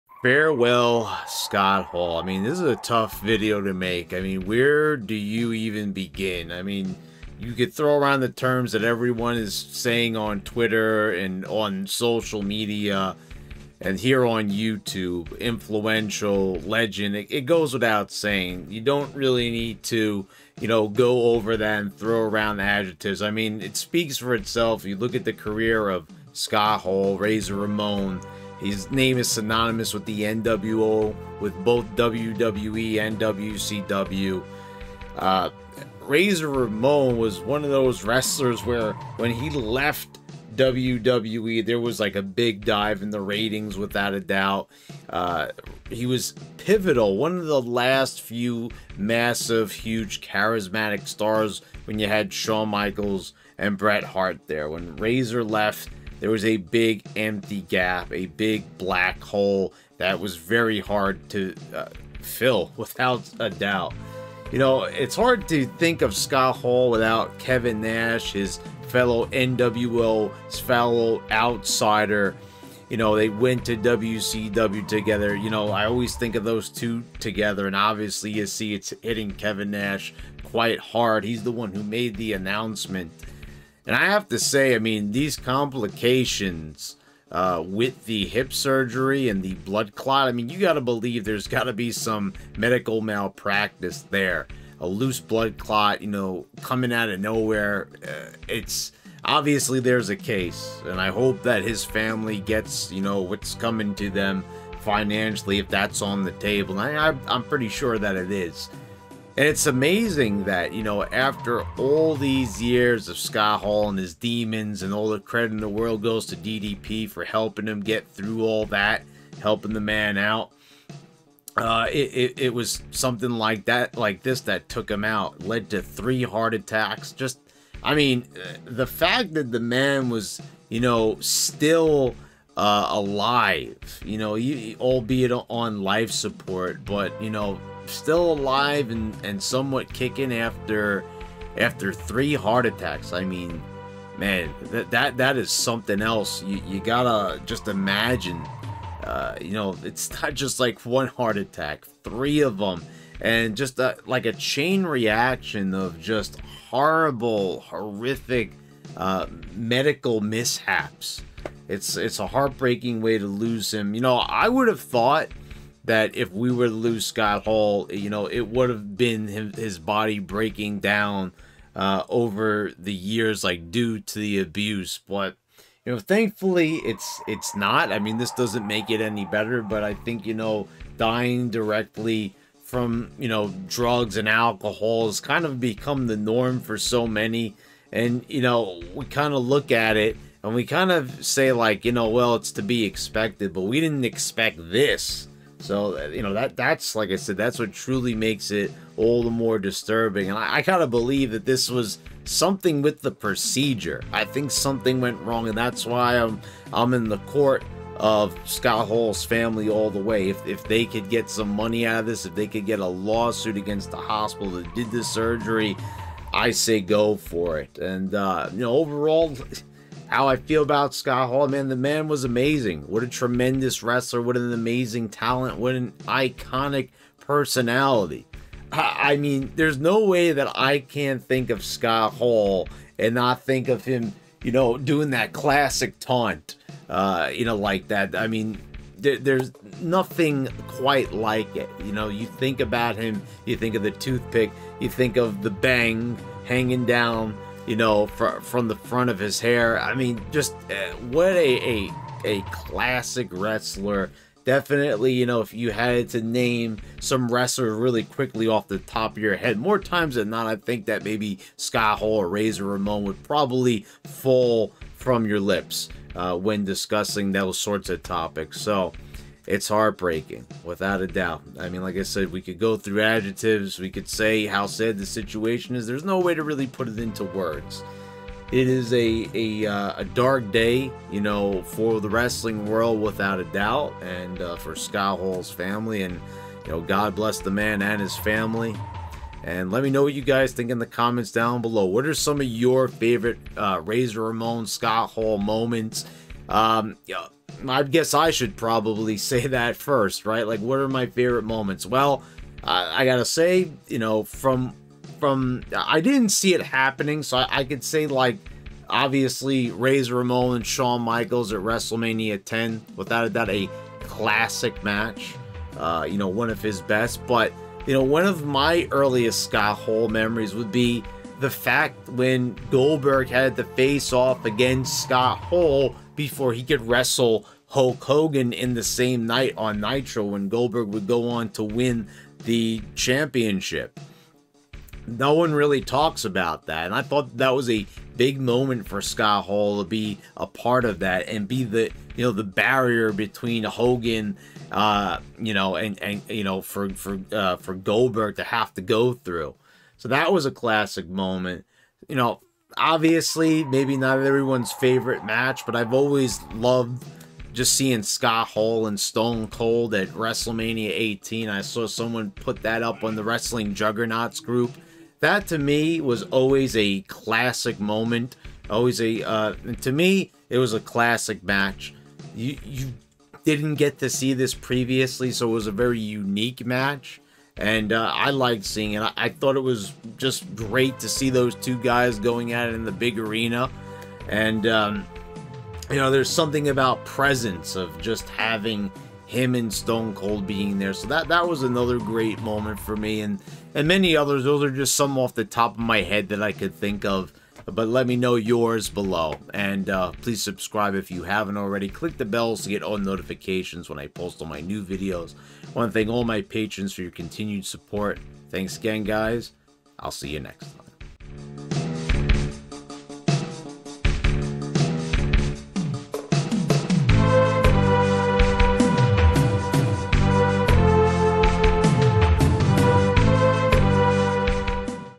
Farewell, Scott Hall...
Last night, we lost one of the greatest of all time, Scott Hall. What happened exactly and what are my thoughts on the whole situation? Let's have a moment to discuss and share some fond memories of this undeniable legend.